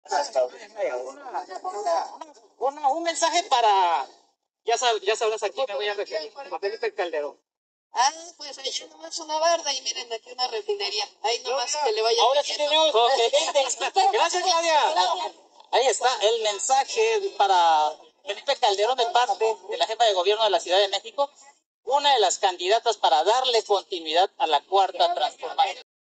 ¡SE QUIERE HACER LA CHISTOSA! CLAUDIA SHEINBAUM INTENTA HUMILLAR A FELIPE CALDERÓN PERO NO LE SALE!
La Jefa de Gobierno de la CDMX queriendo contar chistes, pero ni eso le sale